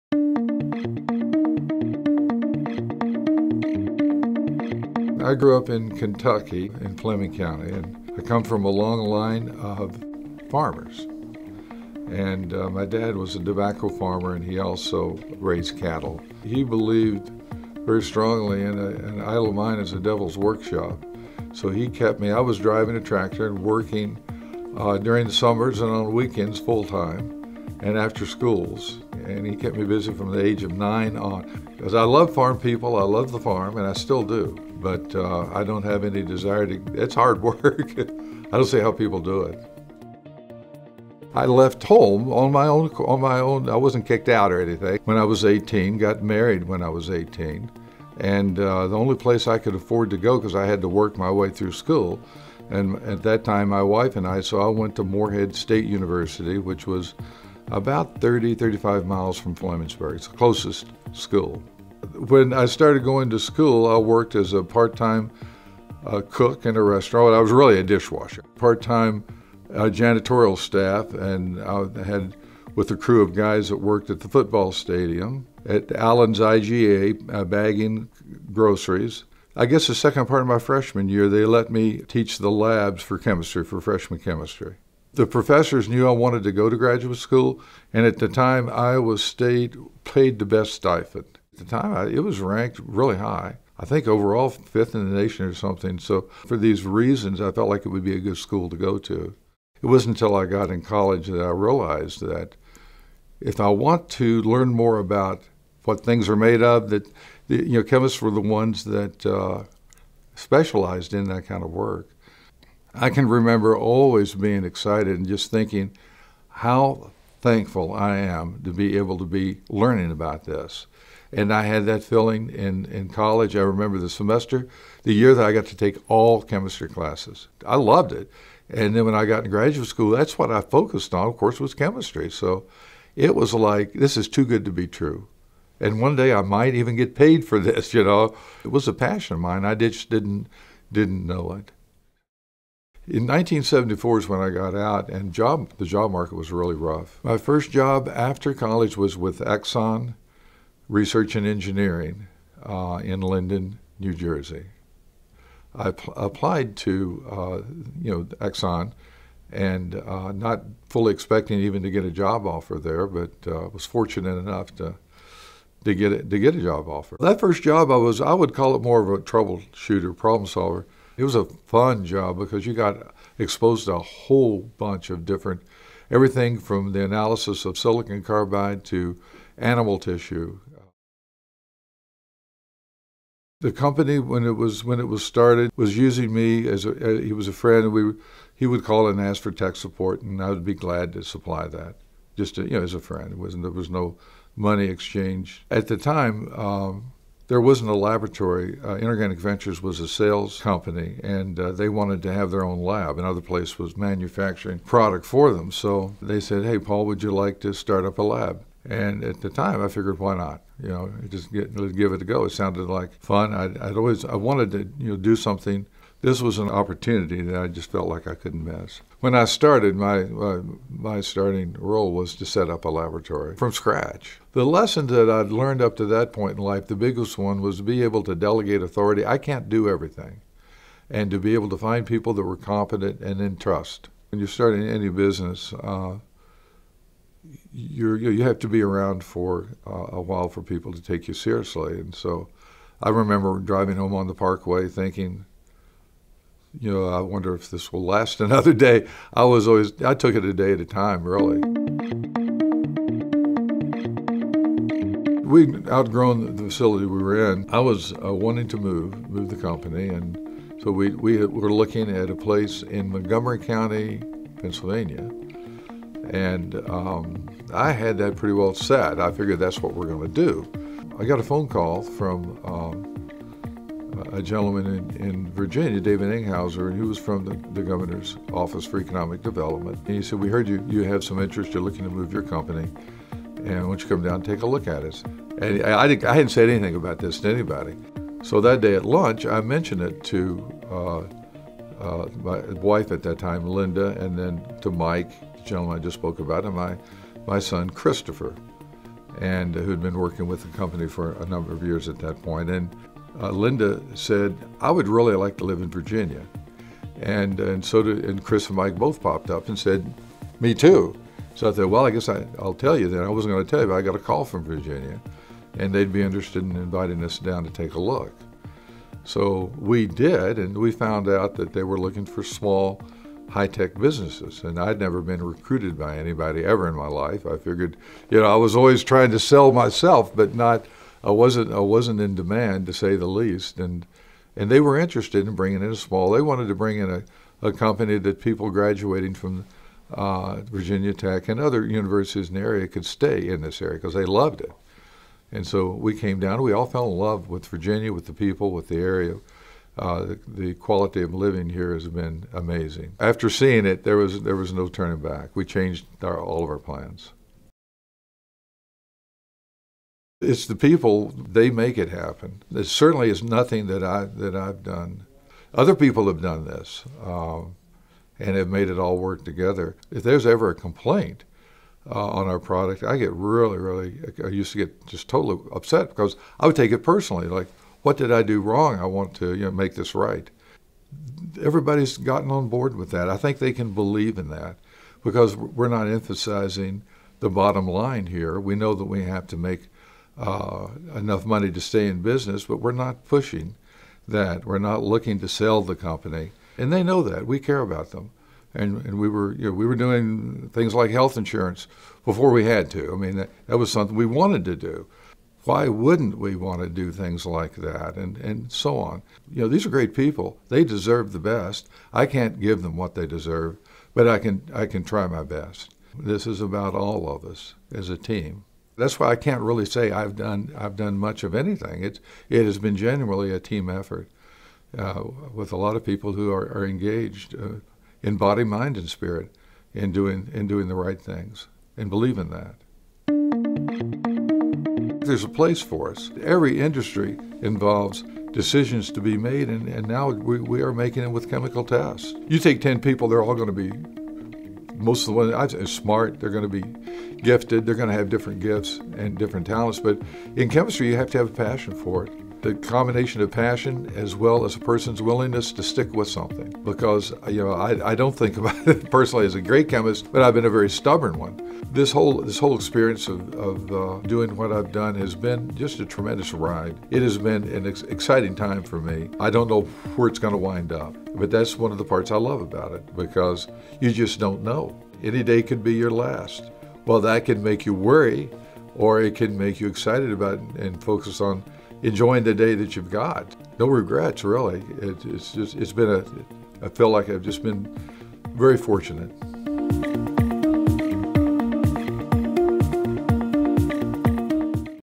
The Making of Inorganic Ventures
Dr. Paul Gaines talks about his career and the growth of Inorganic Ventures.